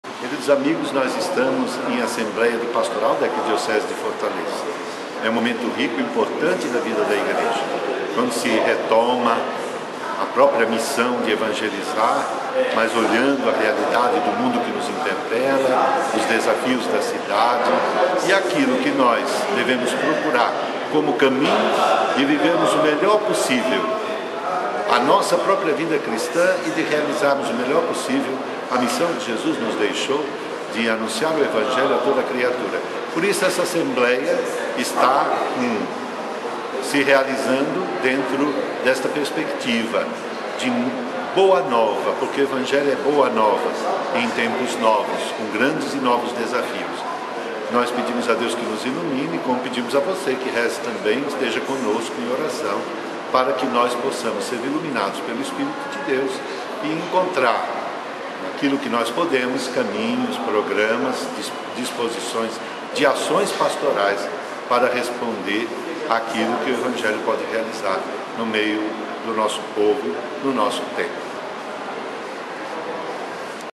dom jose antonio.wmv
Palavra de Dom José Antonio, arcebispo metropolitano de Fortaleza, no início da XXI Assembleia Arquidiocesana de Fortaleza.